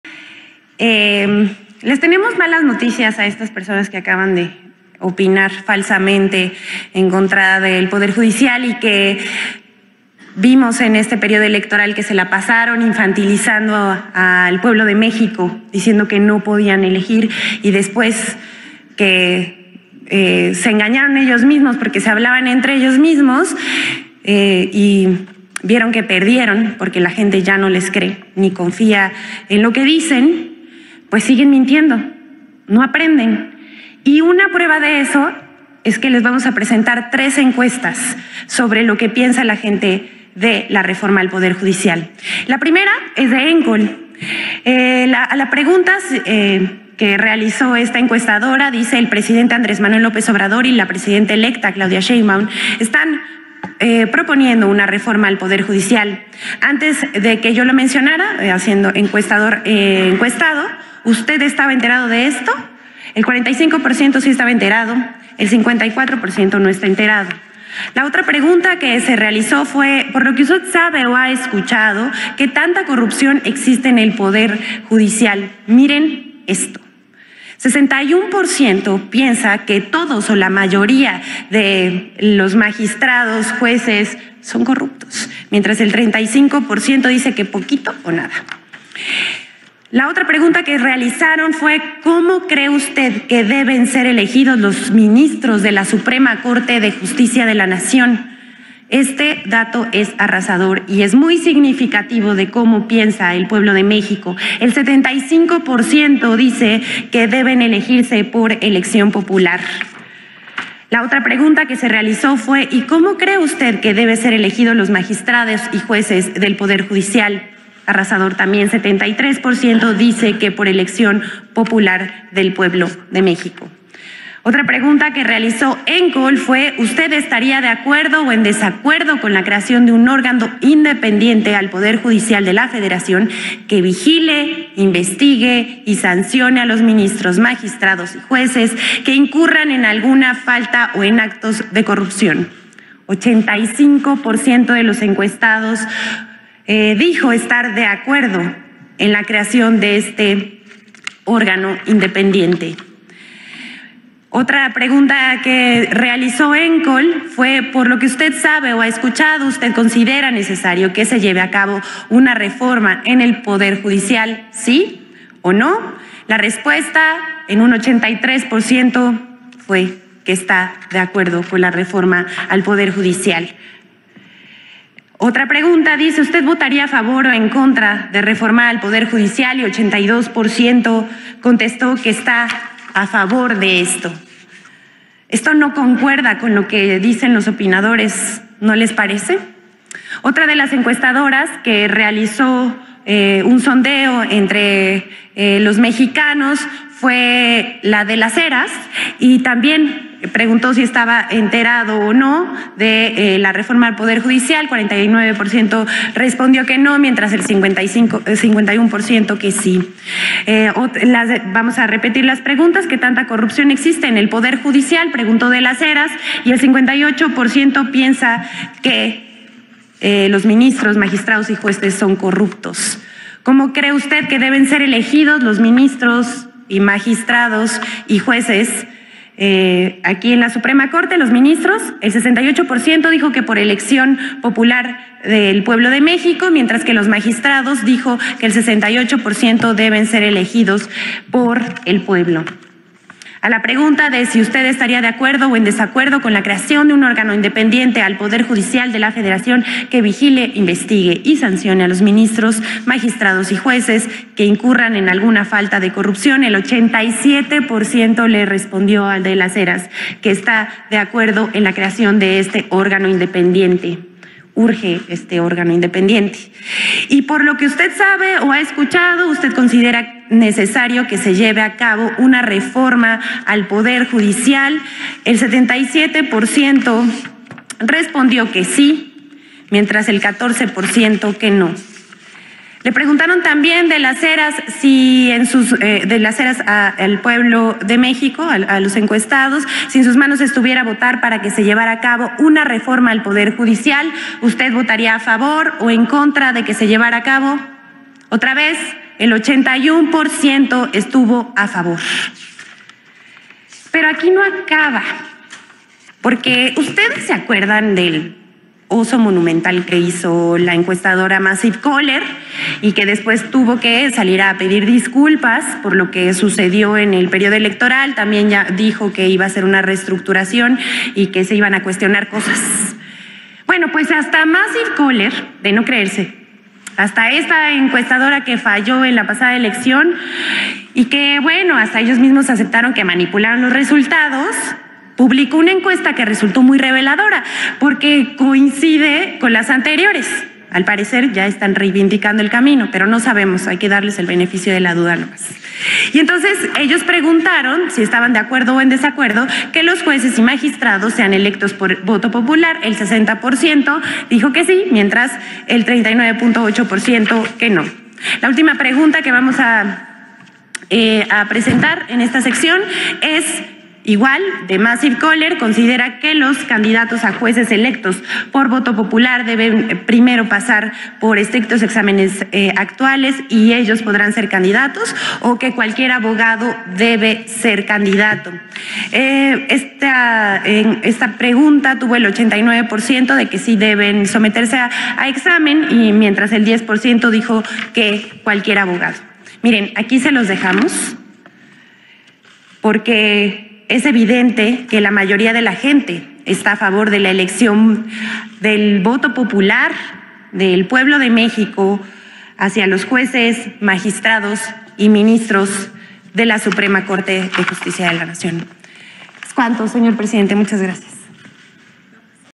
MALAS NOTICIAS PARA CHAYOTEROS Y POLITIQUEROS, ENTRE 77 Y 83% DE MEXICANOS QUIEREN REFORMA JUDICIAL
Finalmente, la periodista Ana Elizabeth García Vilchis aseguró en el “Quién es Quién en las Mentiras de la Semana” que les tiene malas noticias a esos chayoteros y politiqueros que mintieron sobre la Reforma Judicial y que se la pasan infantilizando al Pueblo de México, advirtiendo que se hablan a sí mismos pues ya nadie les cree: Presentó las encuestas de dos casas encuestadoras de gran prestigio, De las Heras Demotecnia y Enkoll, que fueron muy precisas justamente en la pasada elección, y que ahora dan a conocer junto con un estudio de la Comisión de Encuestas de Morena que del 77 al 83 por ciento de los mexicanos están a favor de la Reforma Judicial. Destacó que hasta Massive Caller por fin dijo la verdad y publicó una encuesta muy parecida a las anteriores